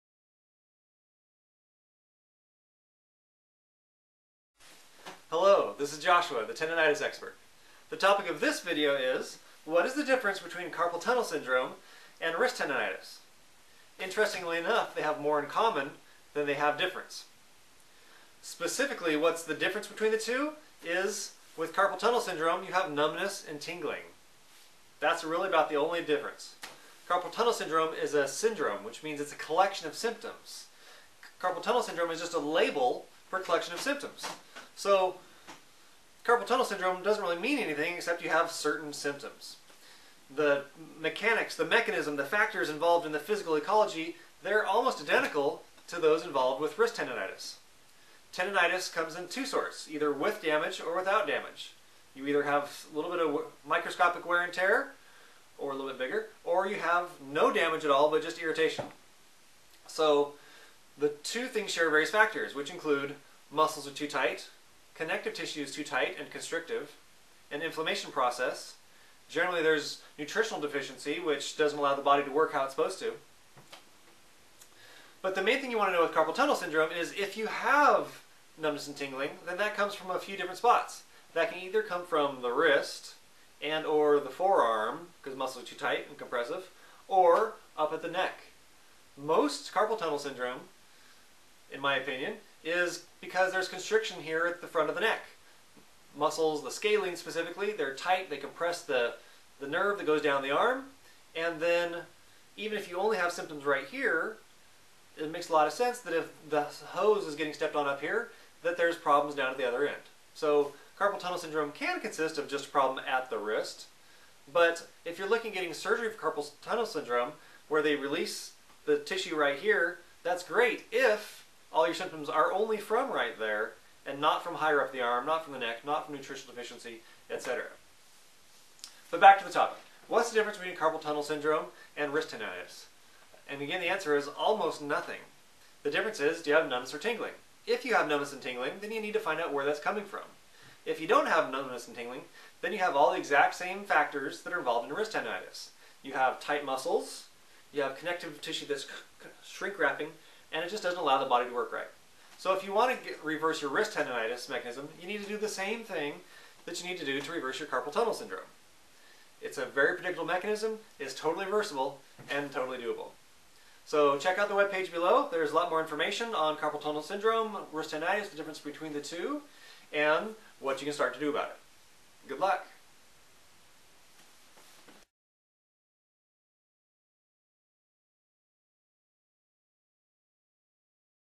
The Difference Between Wrist Tendonitis and Carpal Tunnel Syndrome
What is the difference between Wrist Tendonitis and Carpal Tunnel Syndrome?  Primarily, it numbness and tingling.  Aside from that, they share many of the same qualities.  CTS is also caused by issues at the neck, of course.  But they are each caused by the same Pain Causing Dynamic, the same issues of too tight muscles, too tight connective tissue, an inflammation process, nutritional issues, etc.  To find out more, click on the link above.